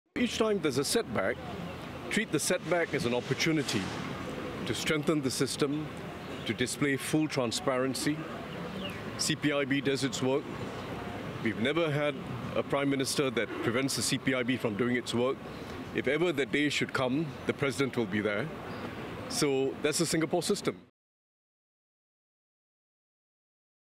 CPIB probe into Iswaran a 'setback' but Singapore system will emerge stronger: Tharman
Weighing in on the Corrupt Practices Investigation Bureau (CPIB) probe into Transport Minister S Iswaran, presidential hopeful Tharman Shanmugaratnam on Saturday (Jul 15) expressed his faith in the "Singapore system" to deal with setbacks and to emerge stronger from it.